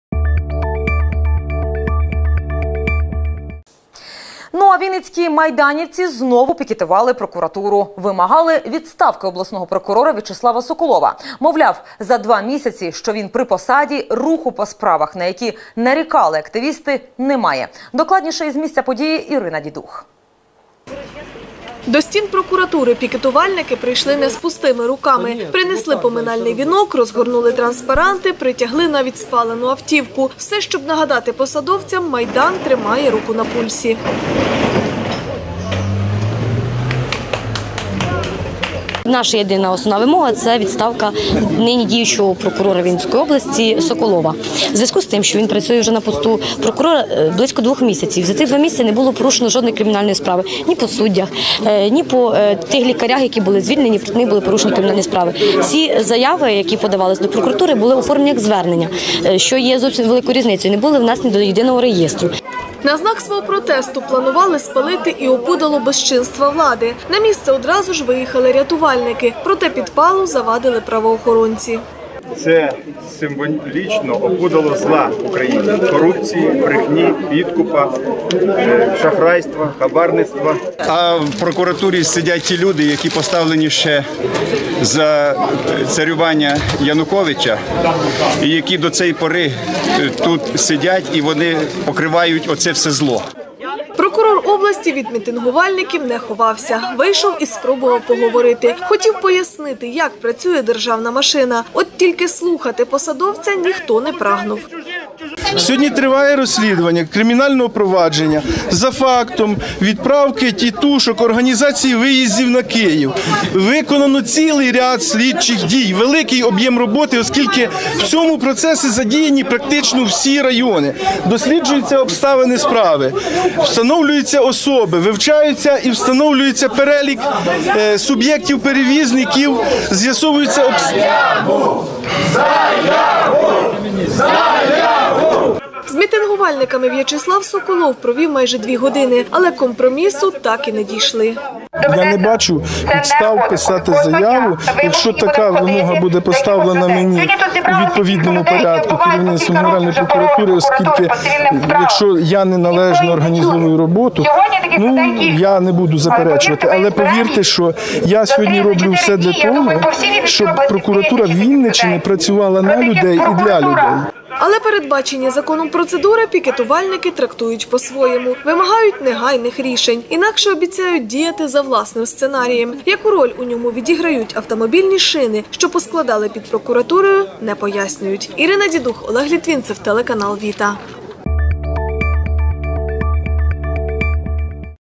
Телеканал ВІТА новини 2014-05-28 Да під Вінницькою, да під прокуратурою
Вінницькі майданівці знову пікетували прокуратуру. Вимагали відставки обласного прокурора В'ячеслава Соколова. Мовляв, за два місяці що він при посаді руху по справах, на які нарікали активісти немає.